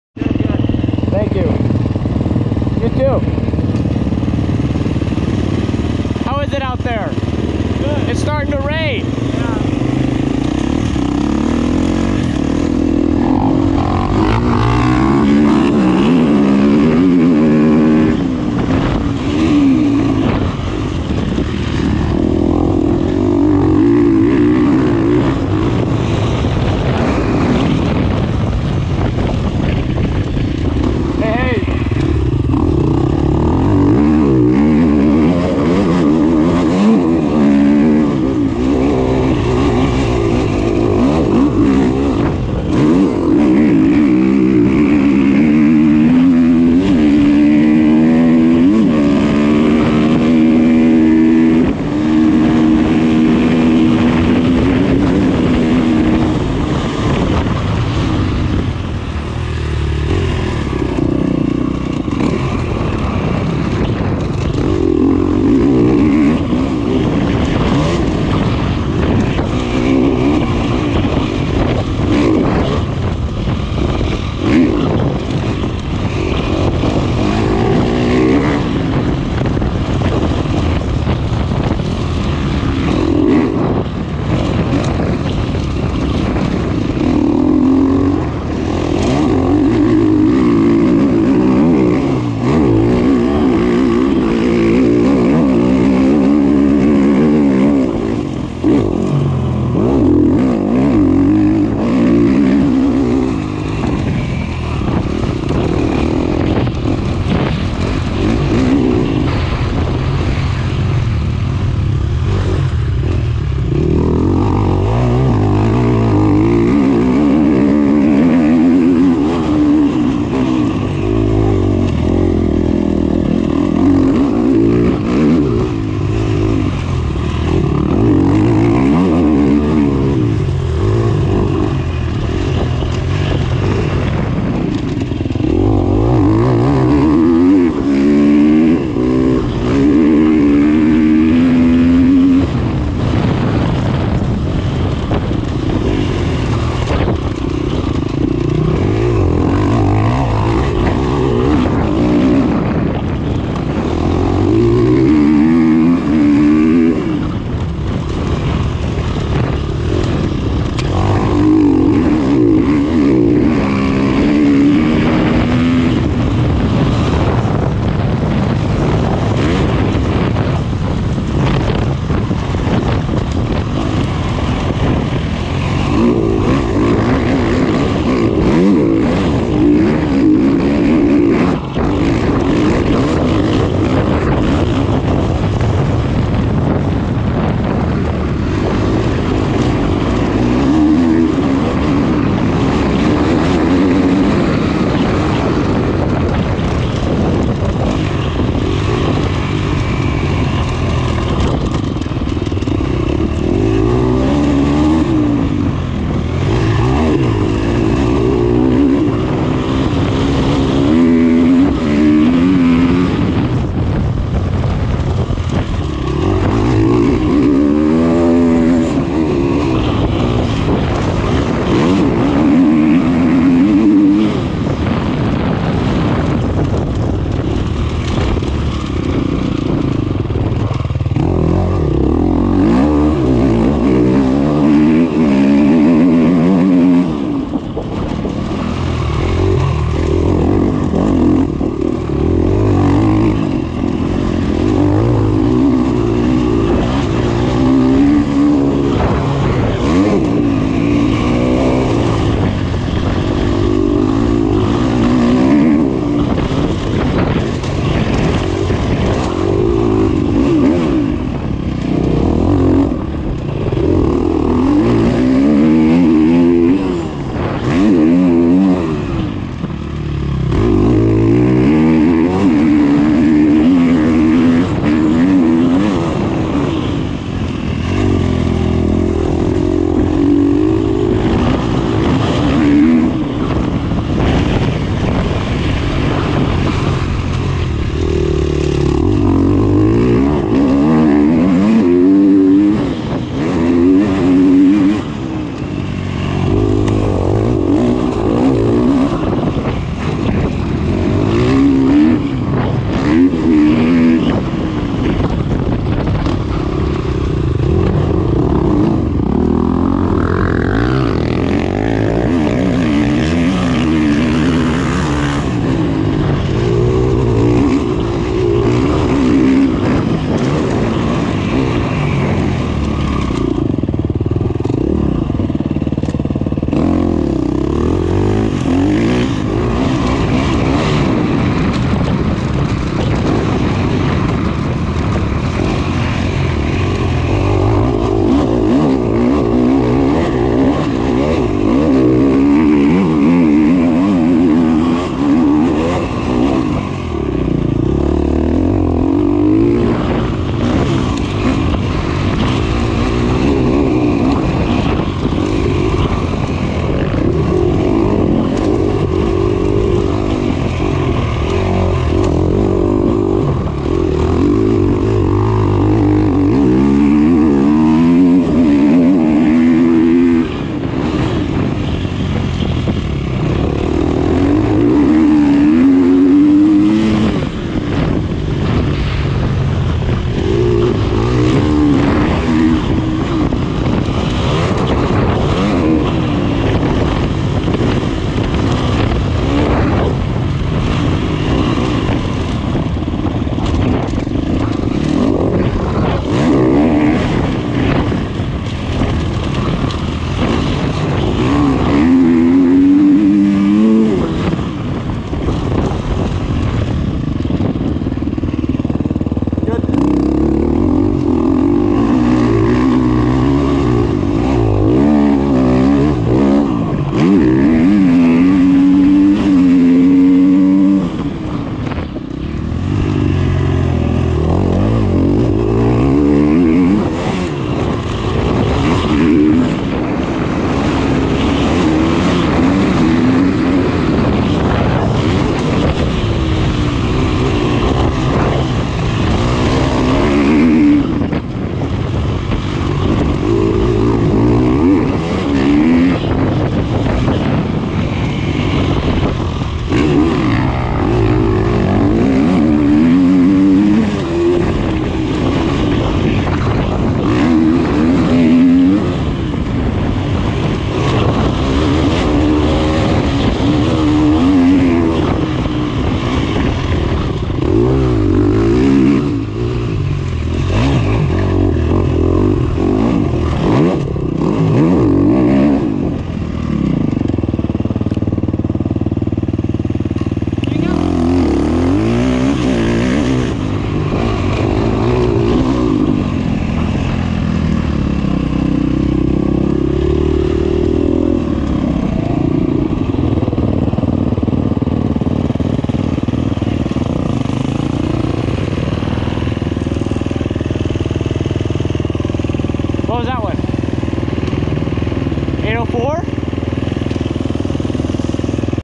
Lewiston GP 2022 (One Lap)
Lewiston GP 2022 Pro Tag team with my brother. Took the dub and had a 8:04 lap time.